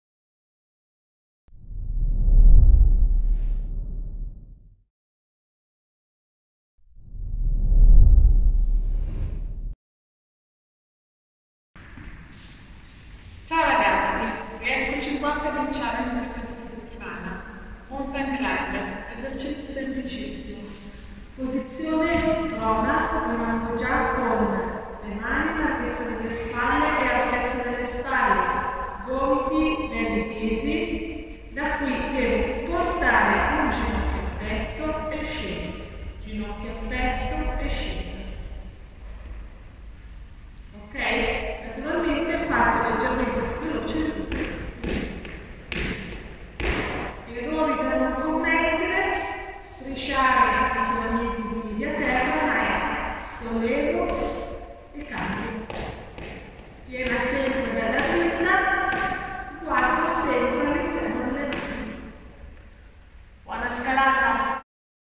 Casnati Challenge | challenge#7 Mountain Climber
Facciamo un po' di movimento!
Un Challenge che abbiamo sottoposto ai nostri studenti affinché ci mandassero le loro performance.
Ma sono esercizi che chiunque può fare e se volete sfidare i nostri ragazzi mandateci i vostri video!
La sezione è in continuo aggiornamento e ogni settimana caricheremo nuovi video.
Centro Studi Casnati Como
Liceo Linguistico Quadriennale
Liceo Linguistico
Liceo Artistico
Istituto Aeronautico
Istituto Alberghiero
Liceo Scientifico Sportivo